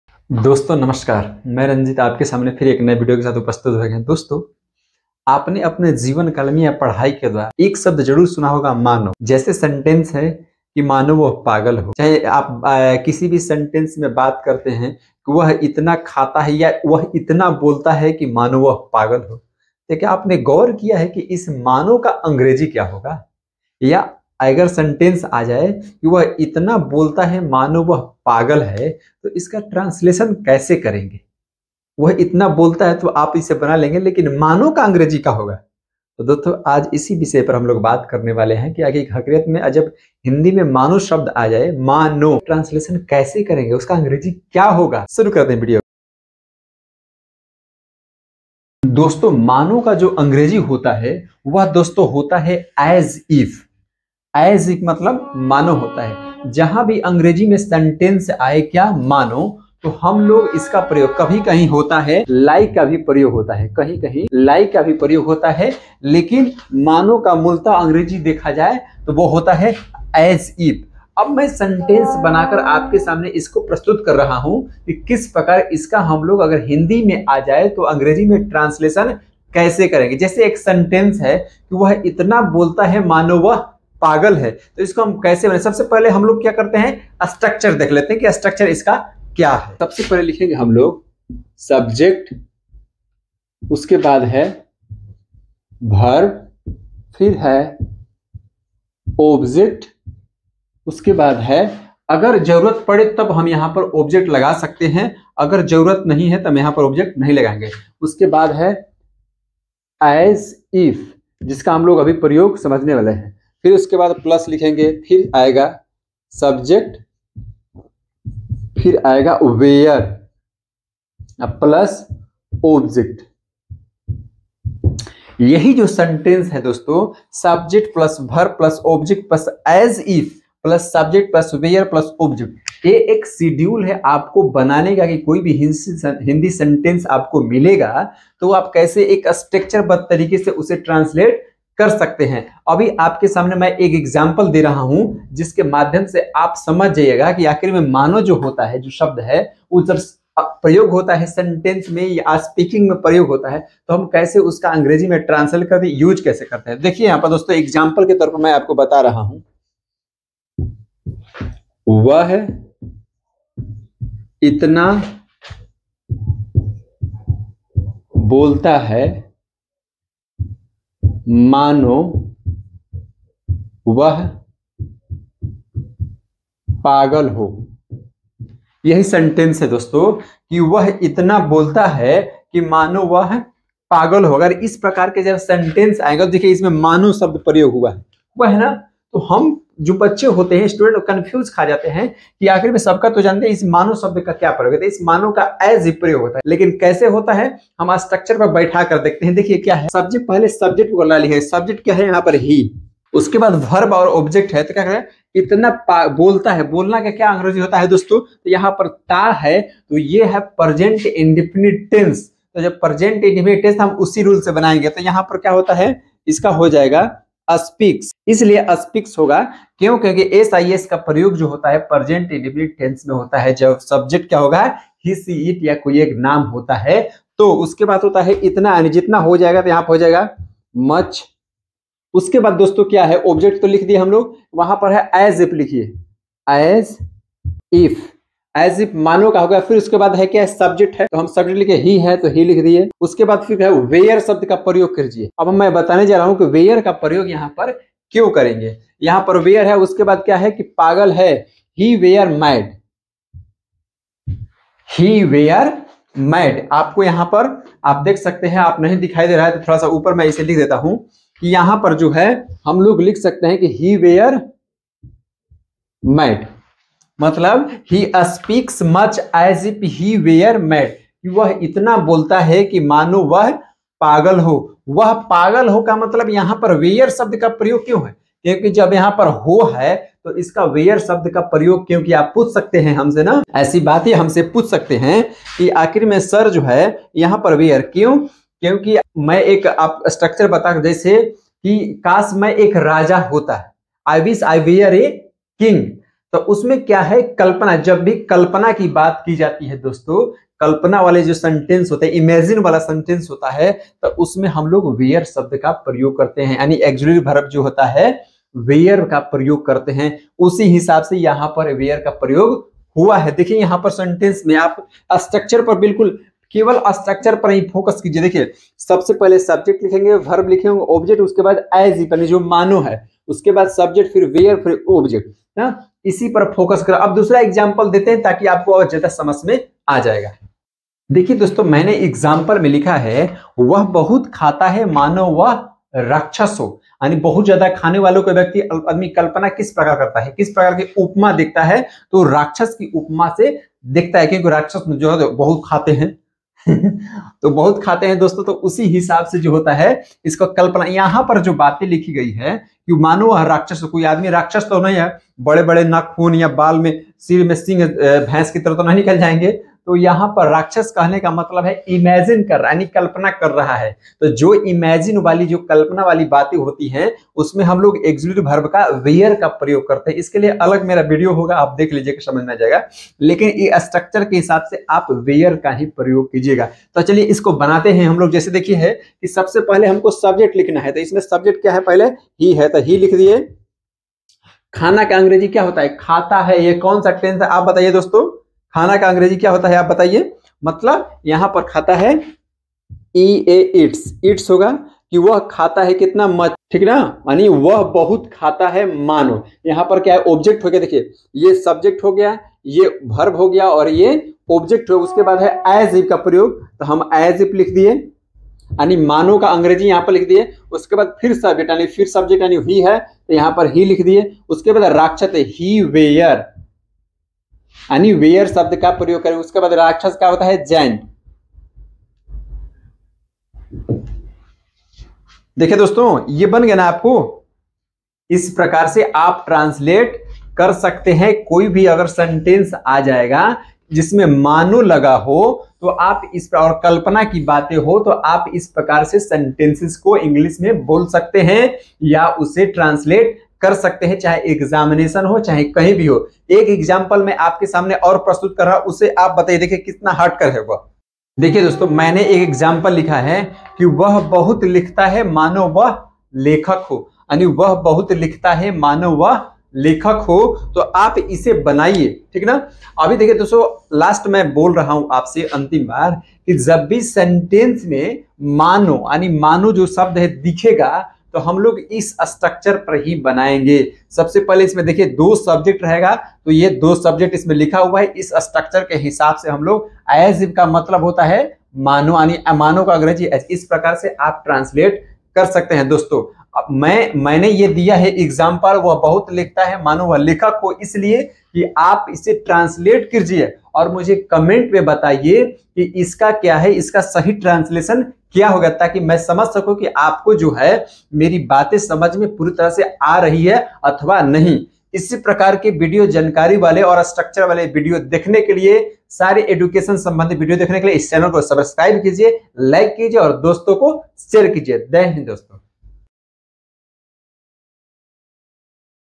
translation।As if(मानो)।मानो वाले वाक्यों का translation ऐसे करें।grammar।English।
इस वीडियो में हम जानेंगे की "मानो " का अगर वाक्य मे प्रयोग हो तो वसे वाक्यों का translation कैसे किया जाए या एक ओर समान्य वाक्य हो दूसरे तरह कल्पना वाले हो तो translation कैसे बनाये।
ranjeet edu point

thanks